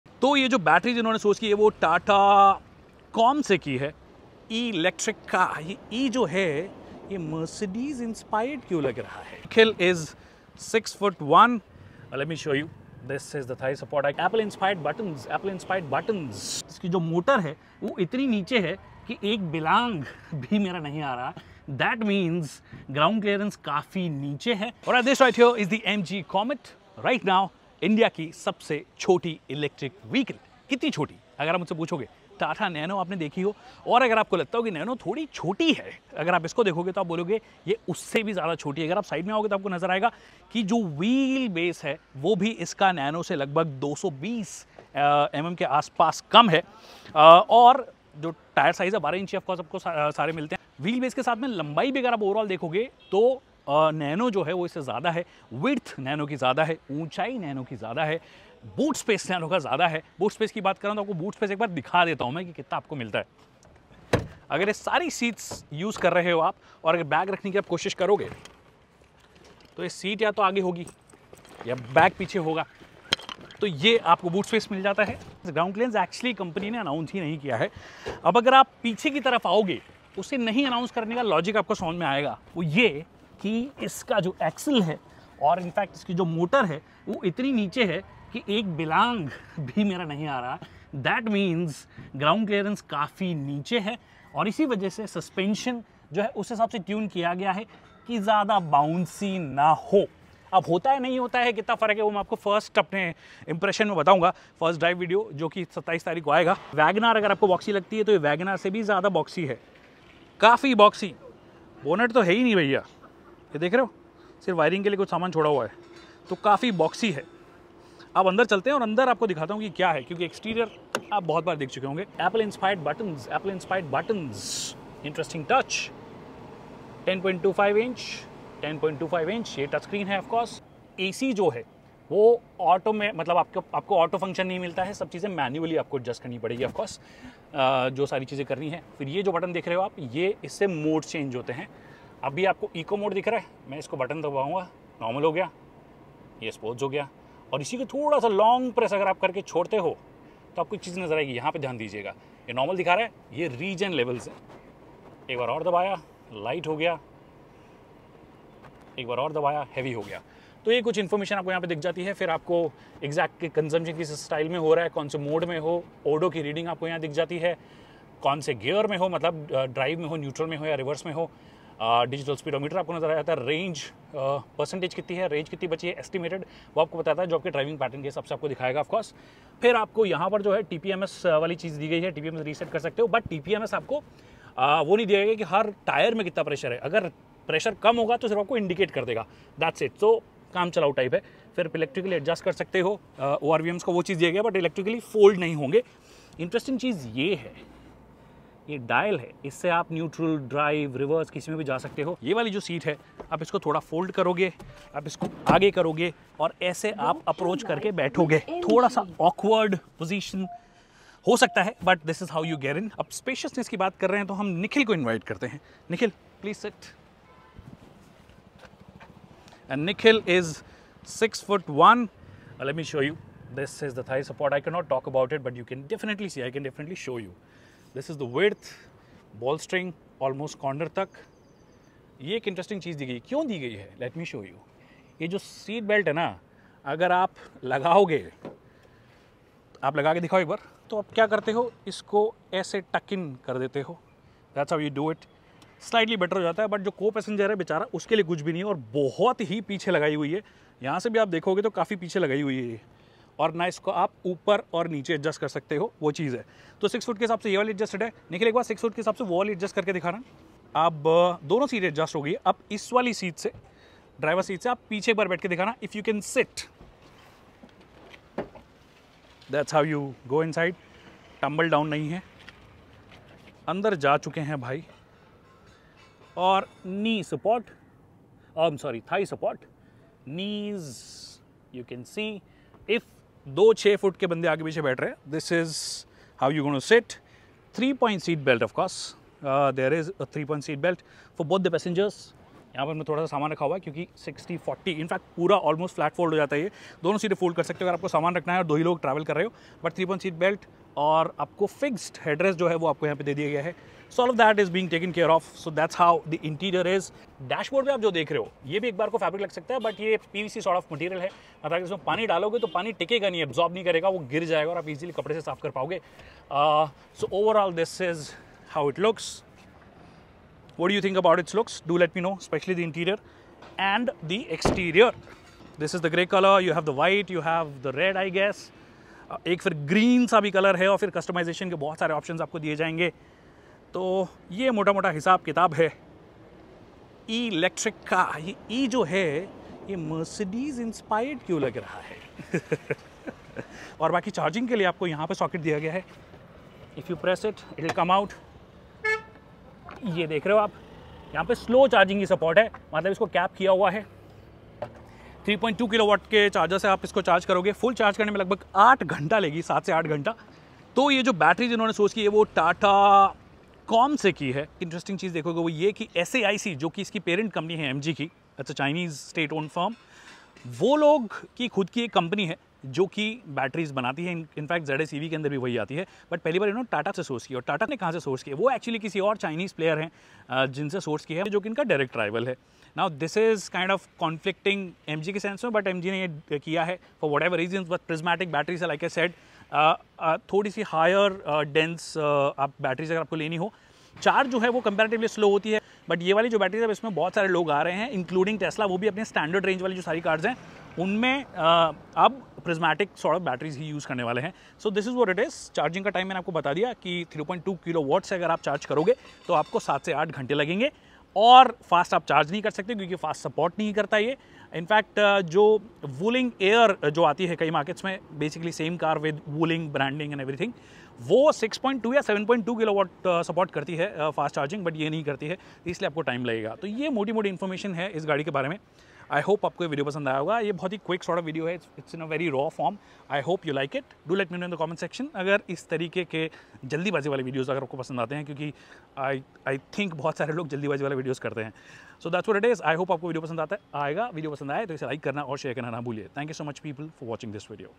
MG Electric Car Comet With Tata's Battery? | First Look | AutoYogi
In our first look video, we tell you everything about MG's upcoming new Micro EV - Comet that aims to attract urban car buyers living in congested big cities of India. Whether it manages to do that or not, only time will tell; we tell you what all it has to offer and most importantly the interior space.